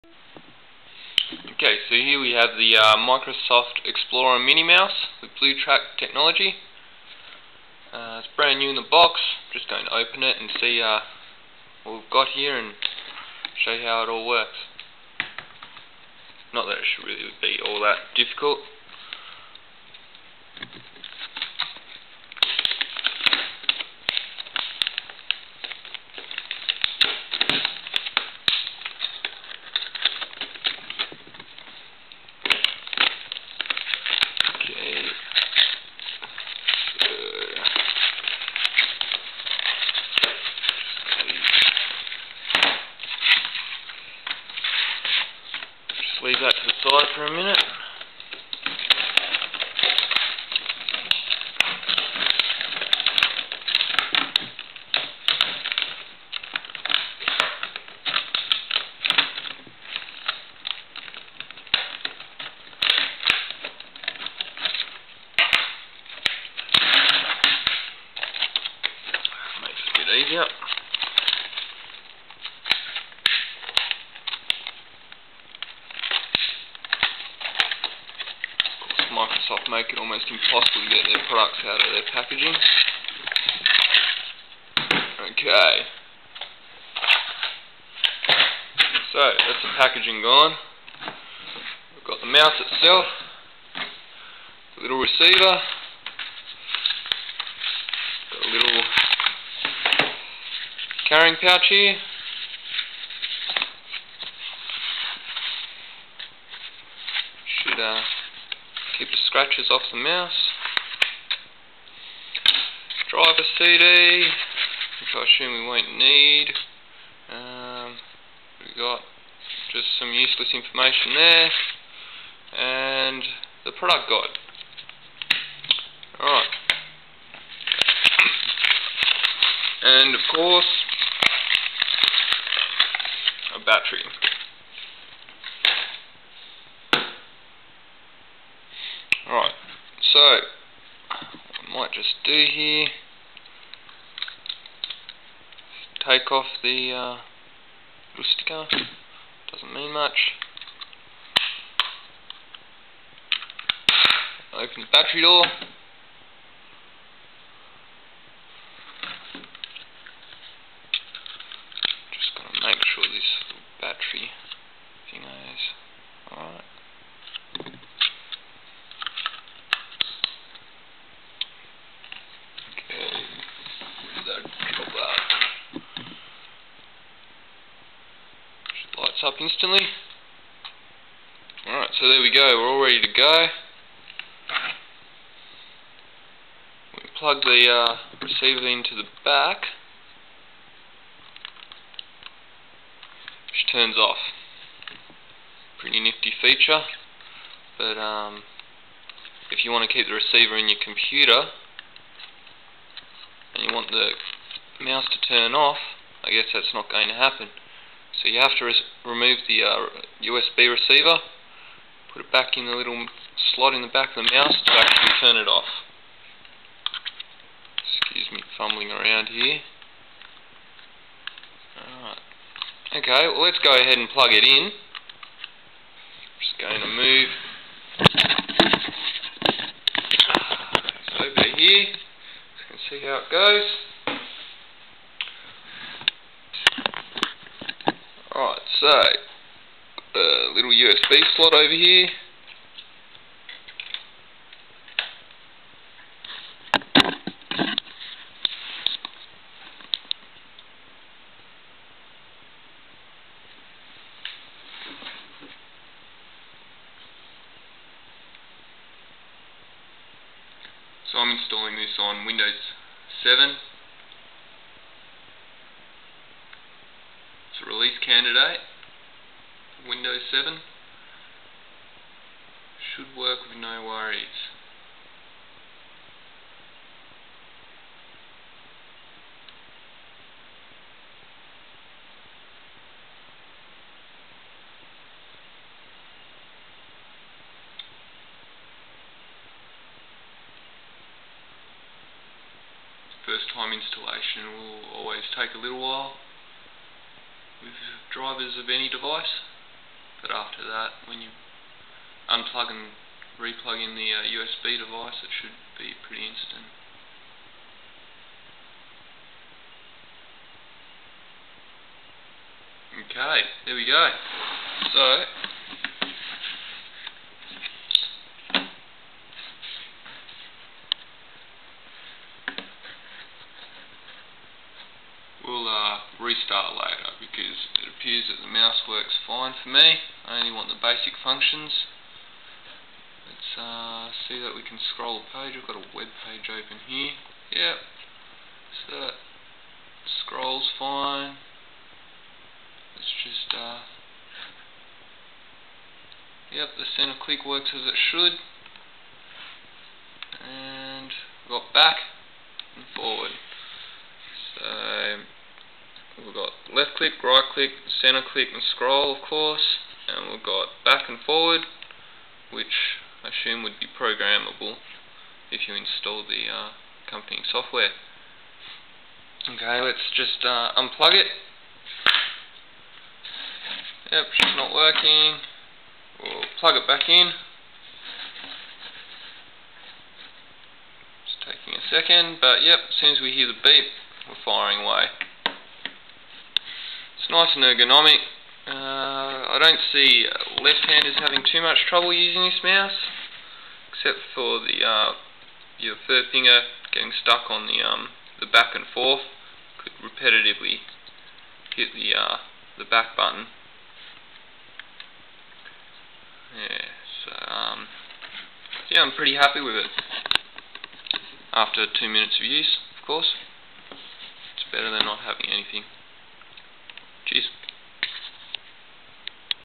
Microsoft Explorer Mini Mouse - Unpacking, Installing and 1st Impressions
Sam unpacks and installs a Microsoft Explorer Mini Mouse.  Showing what's included in the package and outlining a few key details of the mouse.